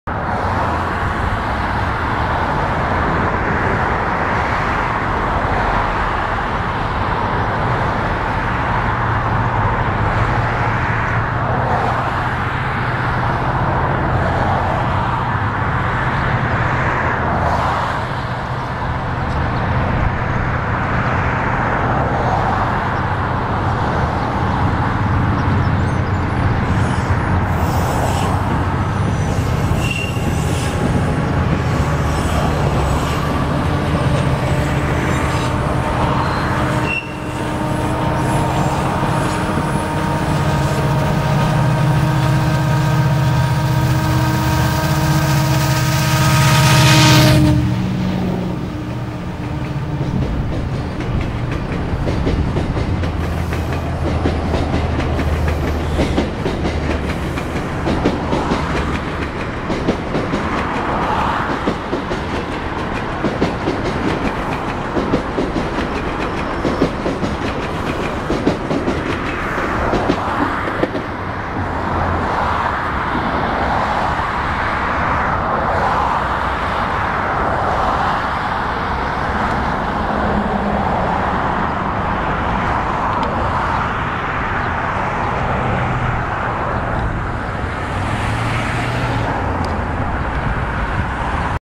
GO 603 Depart Eastbound From Kipling Station
Feb 23-11: We see the last GO train for the day on the Milton Line. 603 is on point as it depart Kipling GO Station, on its way to Union Station